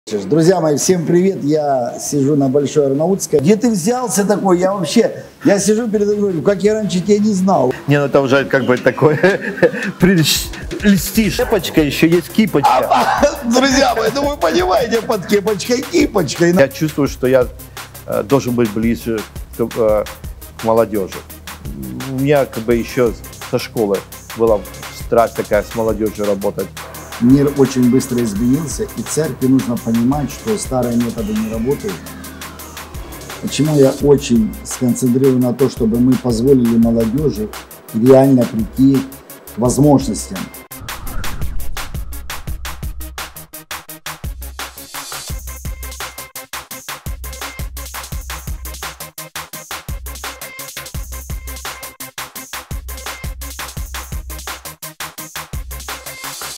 "Диалог на Большой Арнаутской" Размышление по жизни
"Диалог на Большой Арнаутской"
7 июля 2021 г.
В 12:00 (Киев)
Размышление по жизни:
Вера, призвание, настоящее и будущее.
- Истрия жизни или вчера сегодня, завтра
- Модный пастор или достучаться до молодежи.
- какой должна быть церковь будущего?
Ведущий: Валентин Свионтэк (мессианский раввин)
Гость: Сергей Демидович (пастор, телеведущий, автор и исполнитель песен)